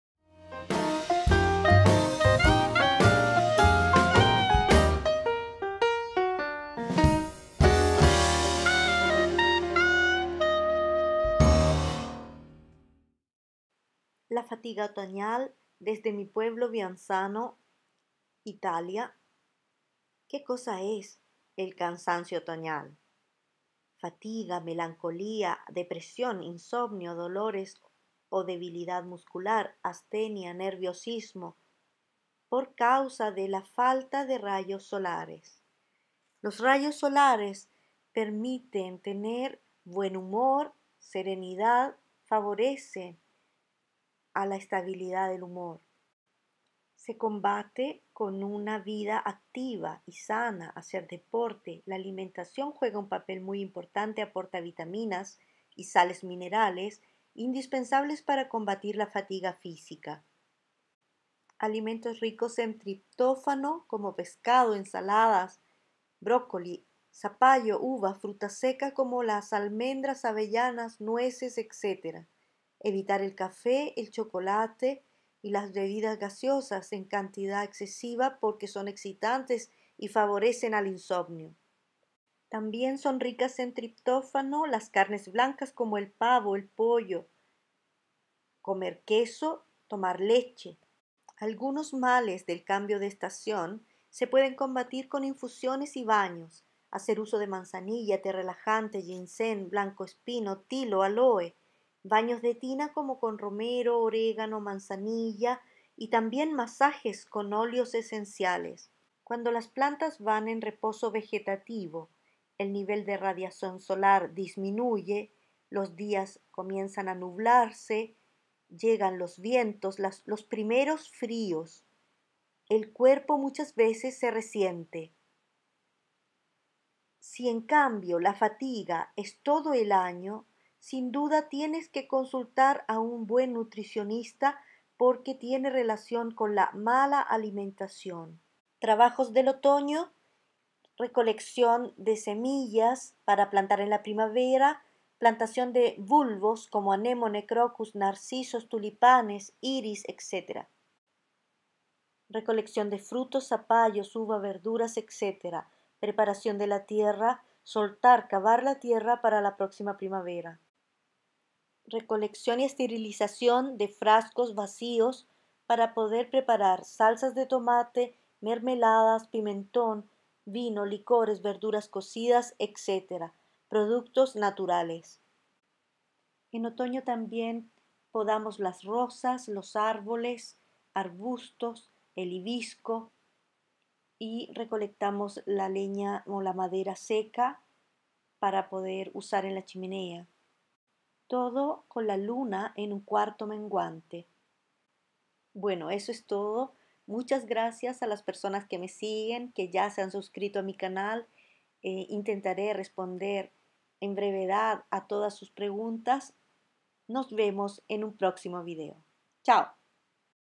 ASTENIA, remedios # 27
Astenia del Otoño, Astenia dell'Autunno, debilidad general de todo el cuerpo, que llega principalmente a un estado crítico en las estaciones frías del año.
 ¿Cómo se cura?, consejos y tareas otoñales.
Vivir Mejor : frutos, flores, hojas, tallos, cortezas, semillas y raíces.  Todo sobre  Plantas y sus beneficios.   Naturaleza, recorridos y pensamientos.